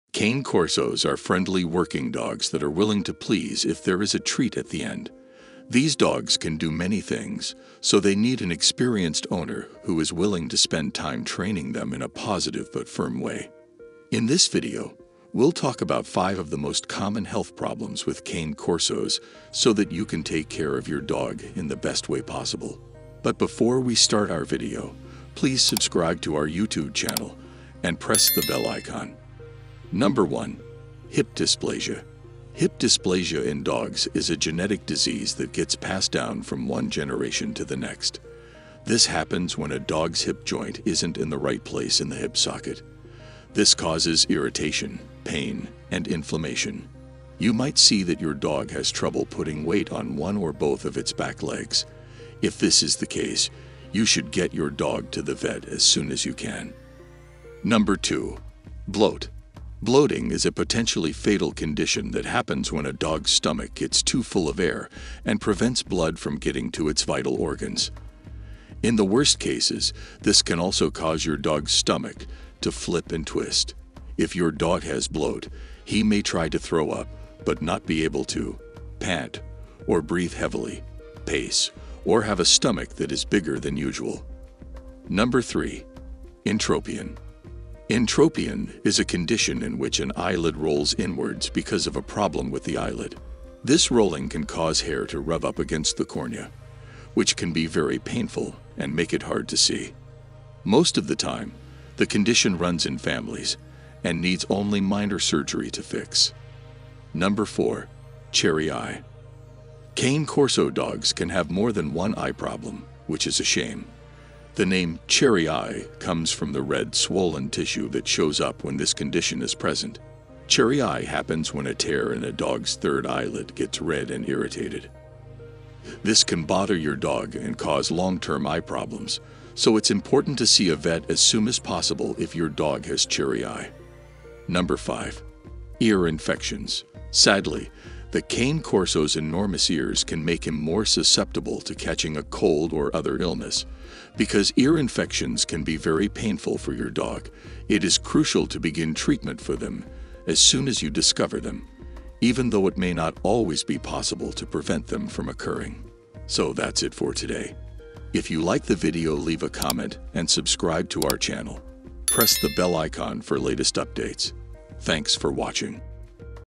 Cane Corso Health Problems: What You NEED to Know
In this informative video, we discuss the top 5 common health problems that Cane Corsos can face. As an owner, it is essential to be aware of these issues so that you can take care of your beloved canine companion in the best possible way. We will cover each health concern in detail, including the symptoms and treatment options available.

Watch this video to learn about:

1. Hip Dysplasia: A genetic disease that affects your Cane Corso's hip joint
2. Bloat: A potentially fatal condition that occurs when a dog's stomach fills with air
3. Entropion: An uncomfortable eye condition that can impact your dog's vision and comfort
4. Cherry Eye: Another eye problem that can cause long-term issues if left untreated
5. Ear Infections: How to Identify and treat ear infections in your Cane Corso

Stay informed and ensure the well-being of your Cane Corso by understanding the health problems they may face. We would love to hear about your experiences and any questions you may have!